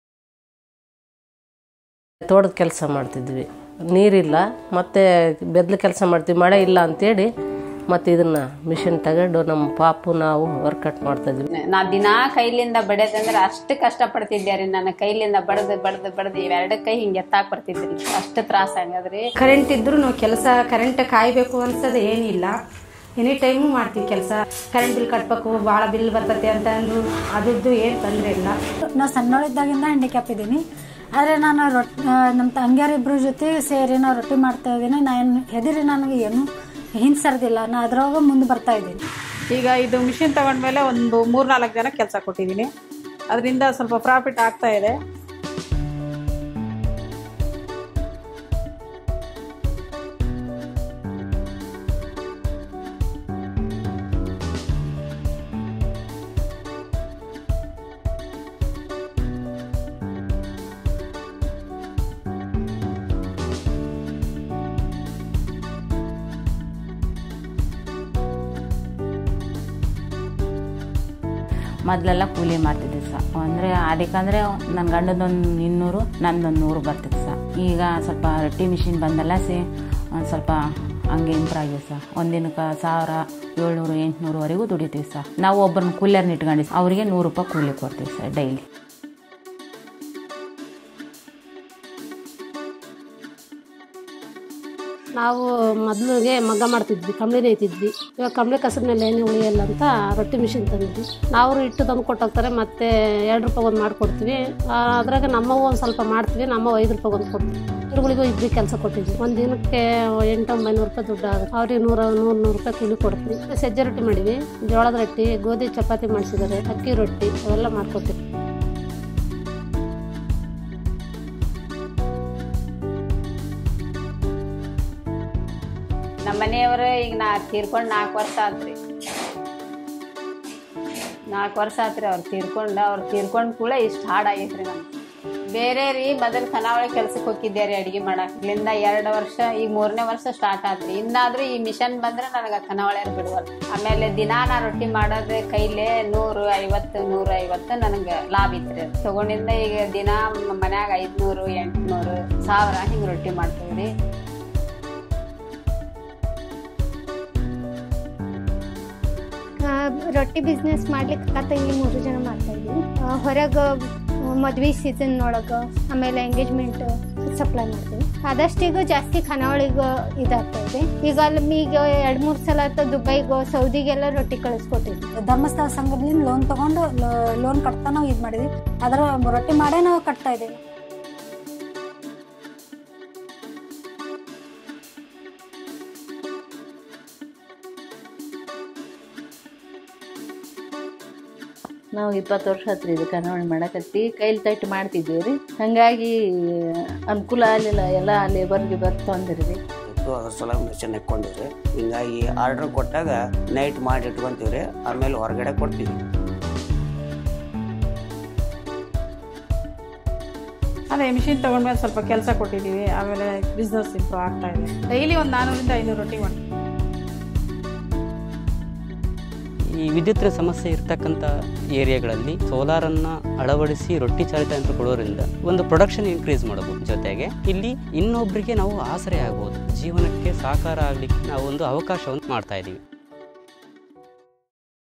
Models and Typologies - Solar Powered Roti Rolling Entrepreneurs
Solar Powered Roti Rolling or Bread Rolling is a unique entrepreneurship opportunity for many people who are looking for new or alternate sources of income. This video captures how different entrepreneurs use the solar powered roti rolling machine for their livelihoods in different ownership models, serving different types of markets.